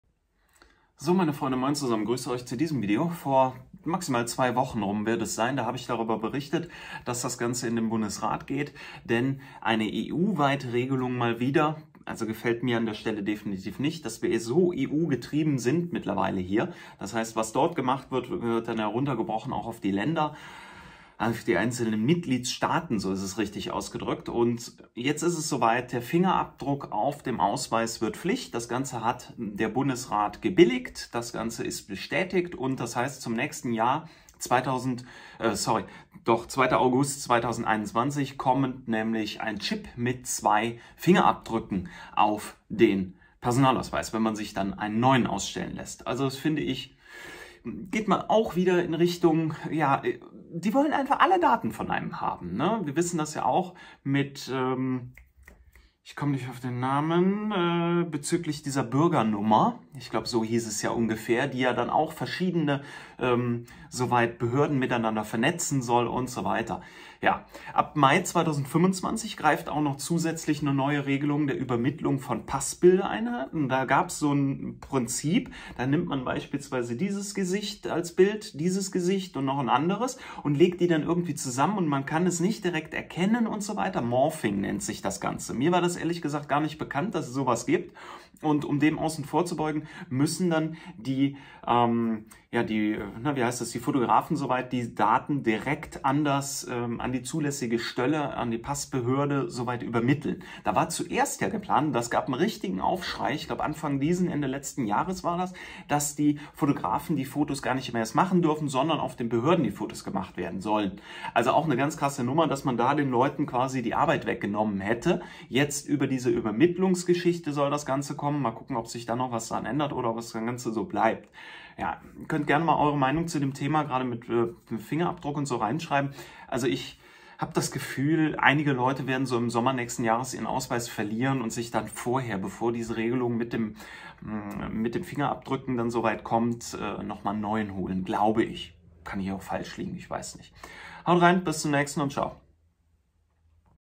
Fingerabdruck auf Ausweis wird Pflicht - Bundesrat bestätigt EU Regelung!
📰 Quellen
Die Personalausweis Fingerabdruck Pflicht kommt, dies wurde nun vom Bundesrat, dass die EU Regelung kommt, bestätigt.